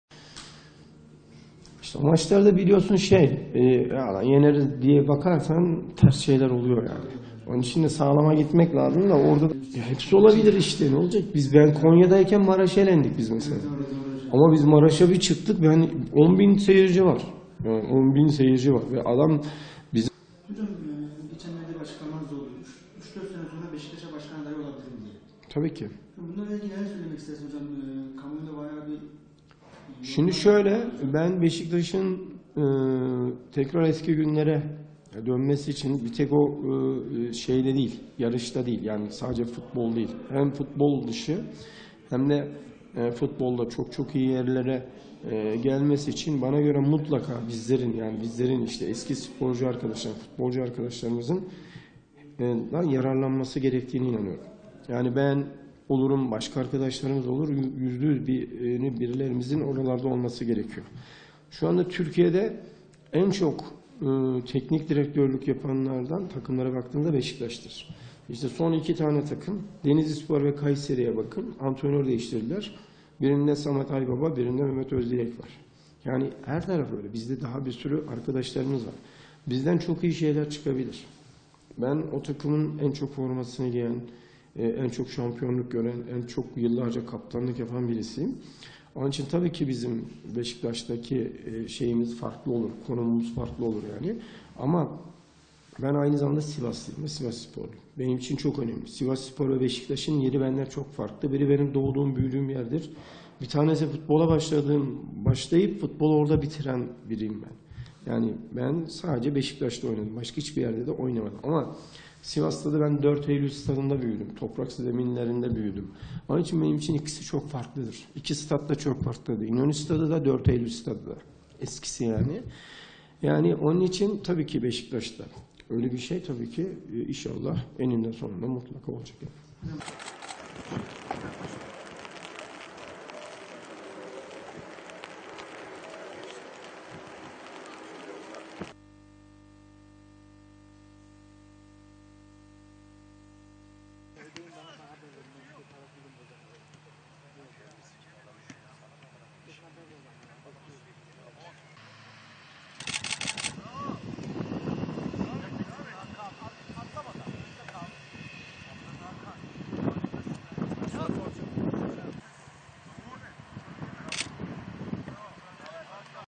Rıza Çalımbay: "Beşiktaş'a başkan adayı olabilirim"
- Rıza Çalımbay: "Beşiktaş'a başkan adayı olabilirim"SİVAS - Sivasspor Teknik Direktörü Rıza Çalımbay, ilerleyen dönemlerde futbolculuk kariyerinde formasını giydiği Beşiktaş'a başkan adayı olacağını açıklayarak, "Tabii ki aday olabilirim.
SİVAS - Sivasspor Teknik Direktörü Rıza Çalımbay, ilerleyen dönemlerde futbolculuk kariyerinde formasını giydiği Beşiktaş'a başkan adayı olacağını açıklayarak, "Tabii ki aday olabilirim. Yani ben olurum, başka arkadaşlarım olur, yüzde yüz birilerimizin oralarda olması gerekiyor. Beşiktaş'ta öyle bir şey eninde sonunda mutlaka olacaktır" dedi.
Sivasspor Teknik Direktörü Rıza Çalımbay, İhlas Haber Ajansı'na açıklamalarda bulundu. Geçtiğimiz günlerde "3-4 sene sonra Beşiktaş'a başkan adayı olabilirim" açıklamasının sorulması üzerine Çalımbay, "Tabii ki aday olabilirim.